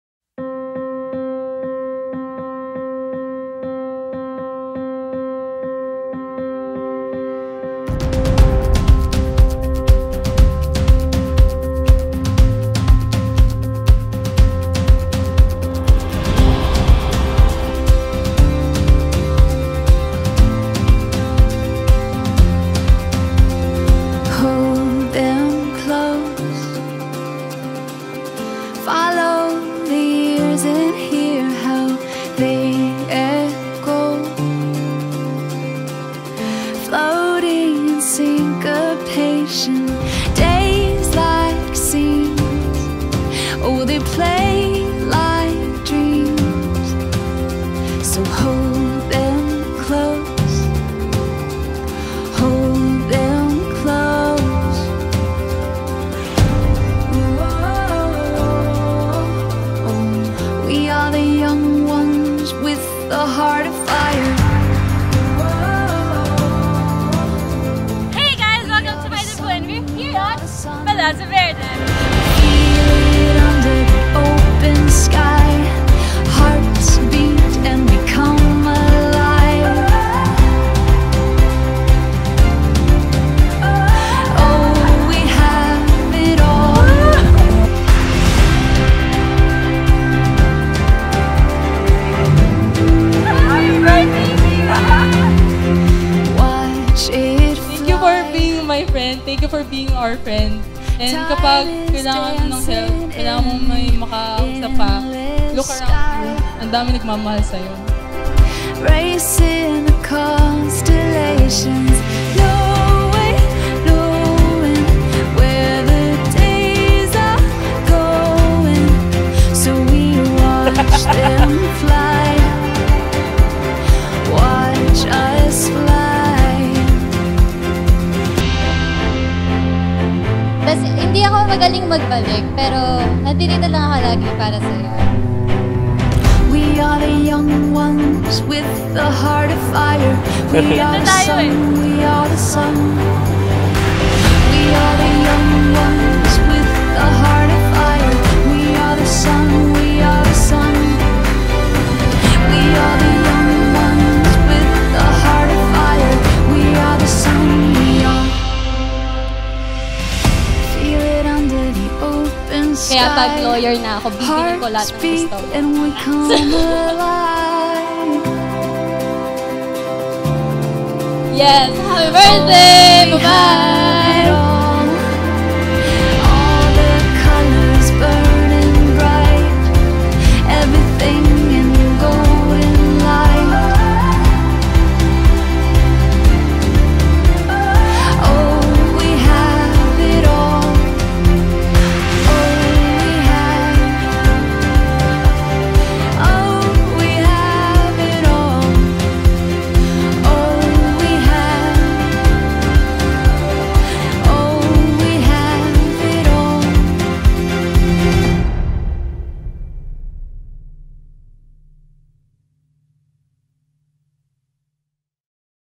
Mira @18 SDE
Mira's debut held at Palazzo Verde, Alabang.